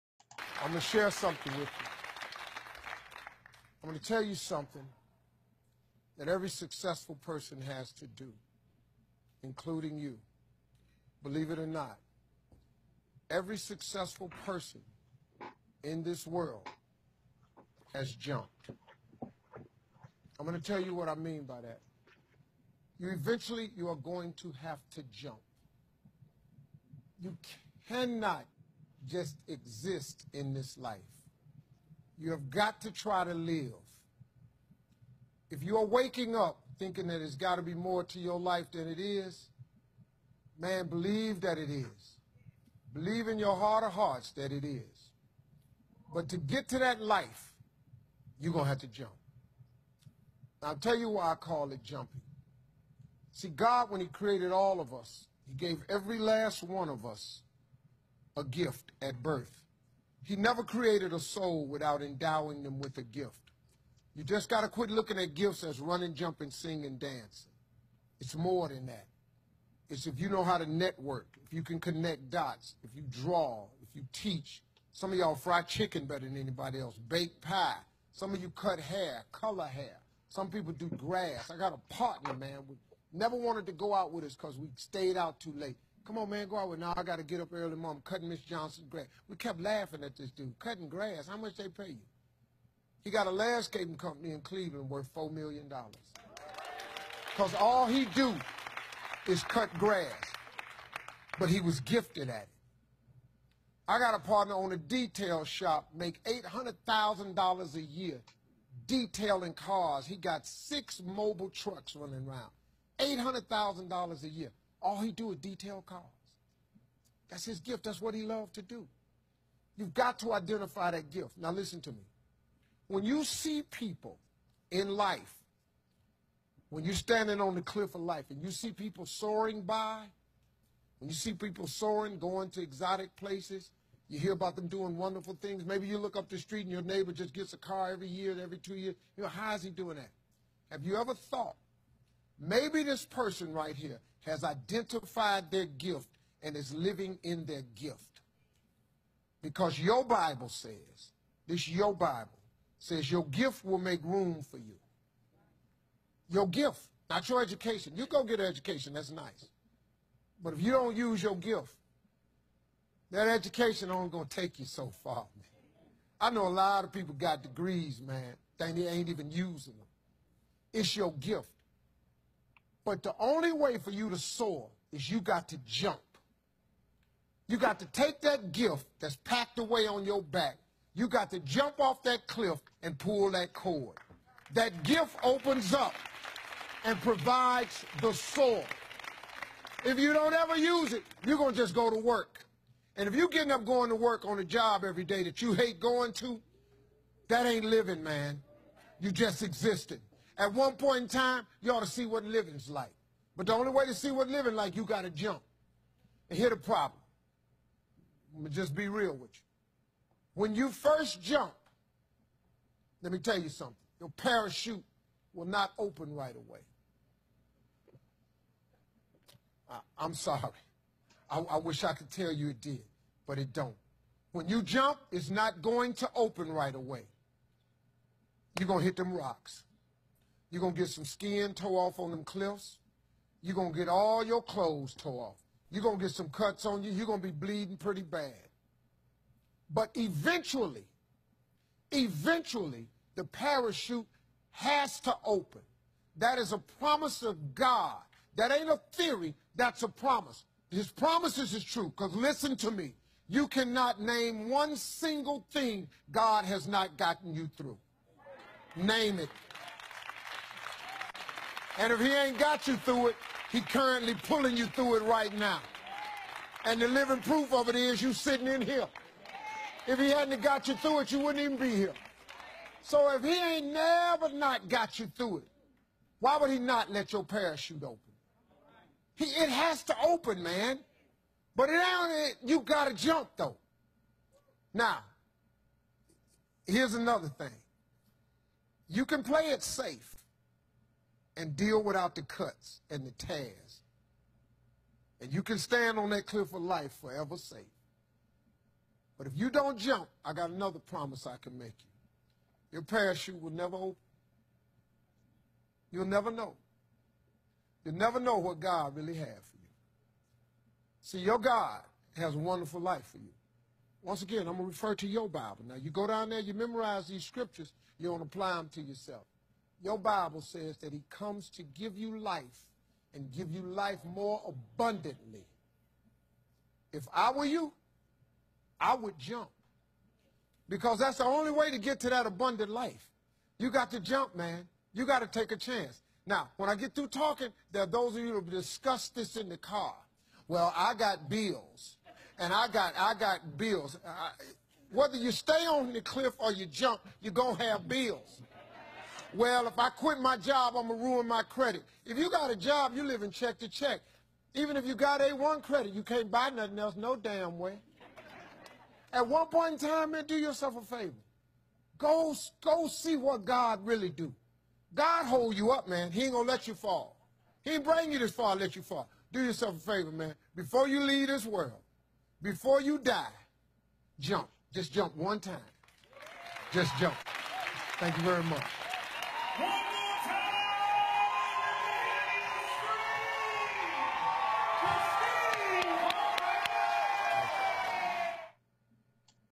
Steve Harvey - you gotta jump After Family Fued taping
After taping a recent episode of Family Feud, Steve Harvey decided to give the audience a sermon on pursuing the gifts God blessed us with and the importance of relying on God in challenging times.